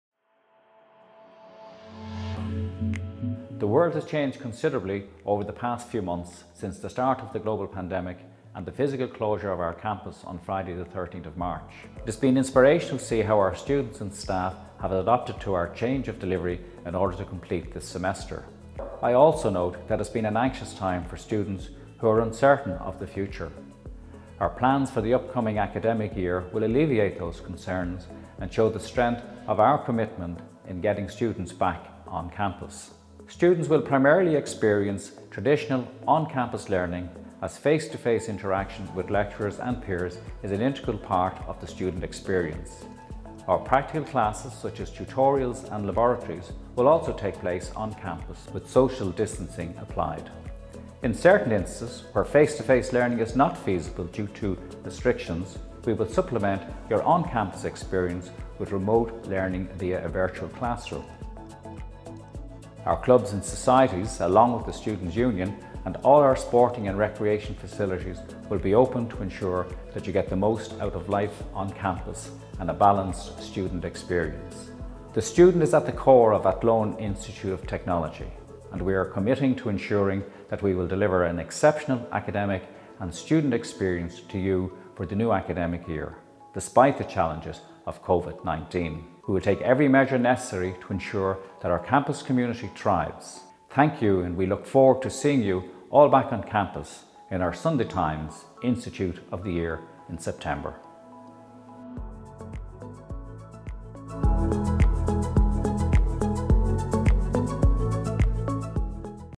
Our Commitment to You | A Message From Our President
AIT President Professor Ciarán Ó Catháin outlines our commitment to you, our students, and the measures being taken to ensure your safety and wellbeing in the coming semester. From social distancing and additional hygiene measures to the adoption of the "virtual classroom", our academic and support staff have been hard at work to ensure you continue to enjoy an exceptional student experience, despite the challenges of COVID-19.